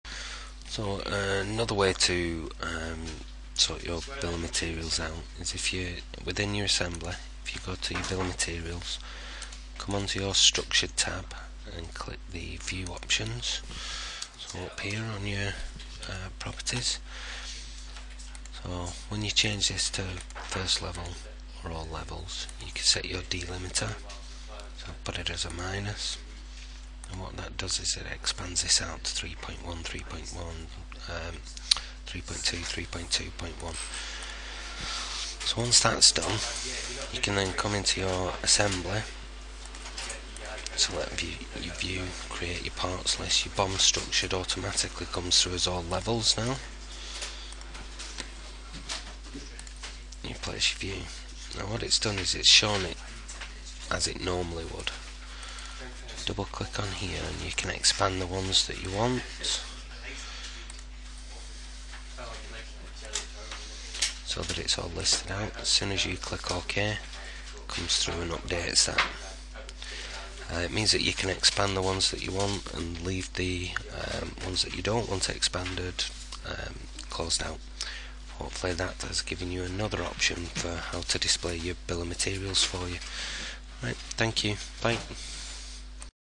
Create a BOM with user selectable multi-level expansion
How to add an expanded bills of materials to a drawing with the ability to expand sub-assemblies if needed.